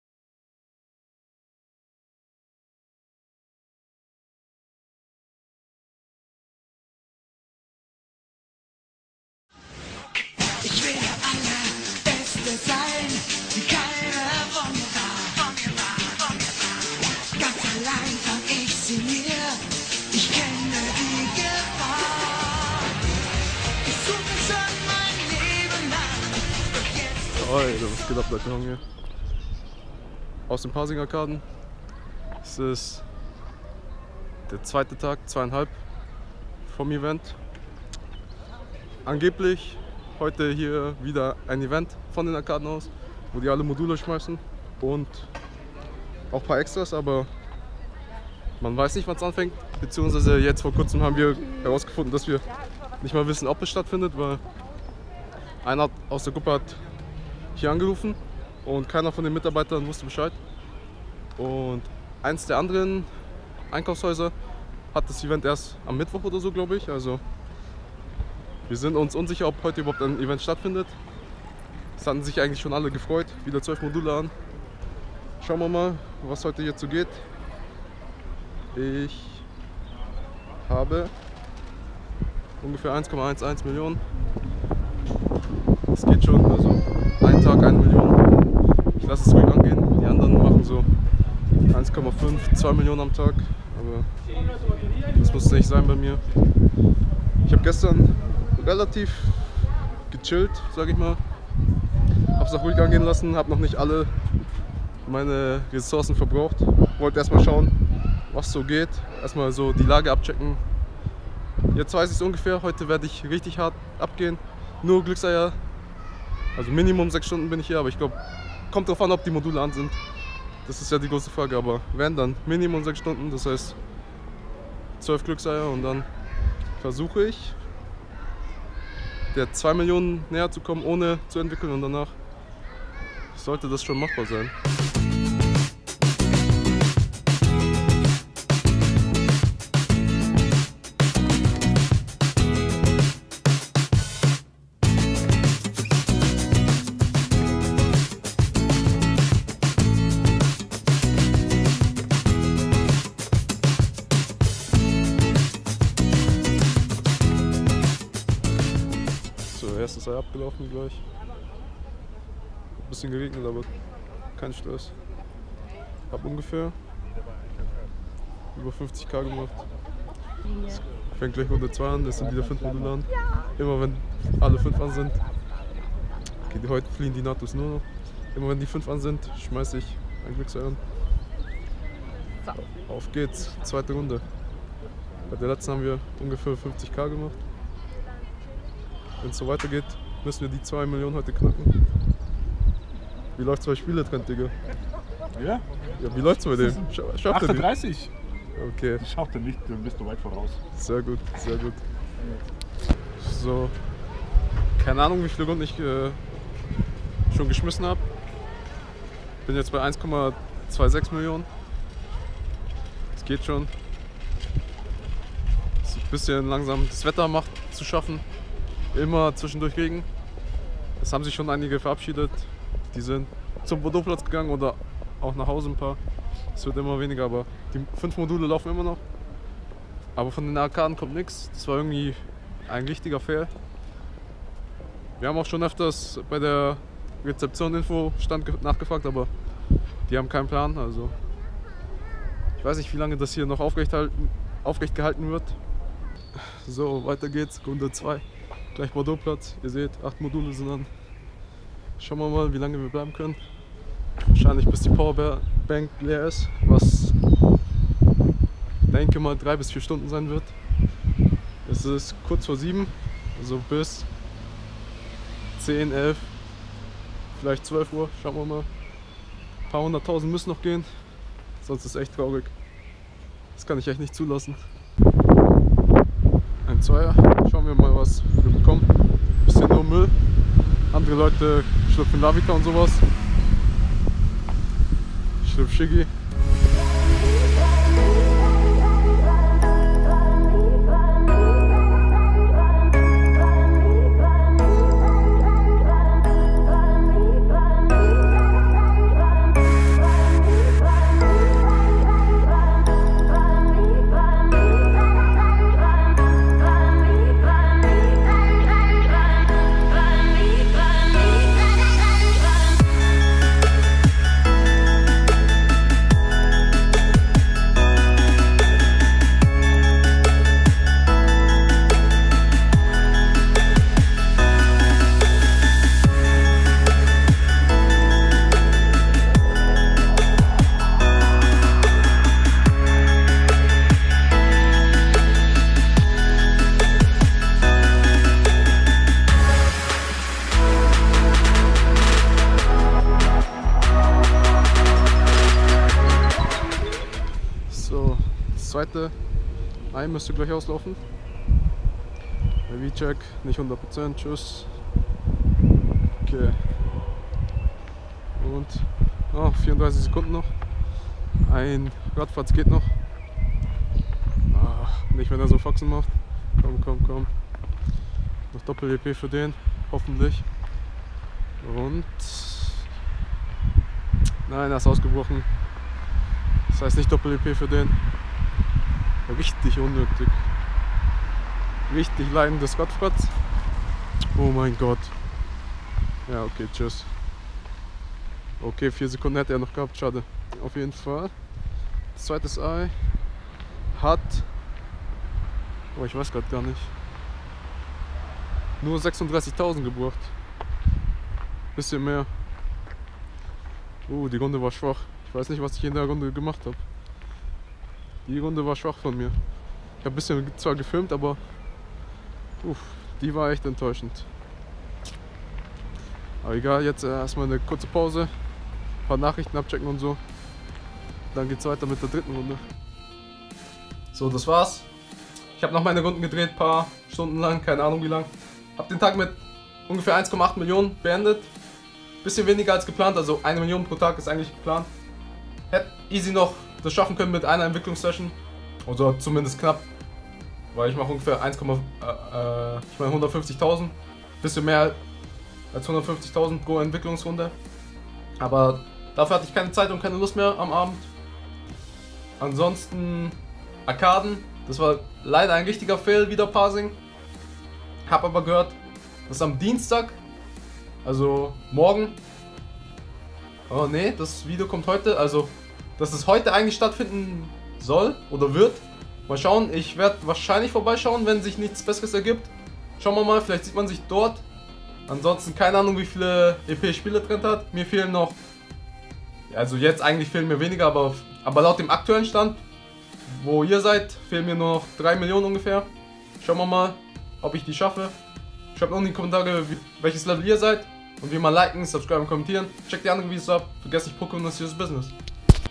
KEINE Lockmodule?! Pasing Event Fail#2! Road to Level 40 - Oster Event Tag 2! Pokemon Go! München
euda Spieletrend hat die Level 40 Challenge angenommen!
wer von uns wird zuerst auf Level 40 kommen?!
zweiter Tag vom Oster Event diesmal in Pasing zu einem angeblichen "Event" dort wieder ... was leider wieder ein Fail war
welches Level seid ihr?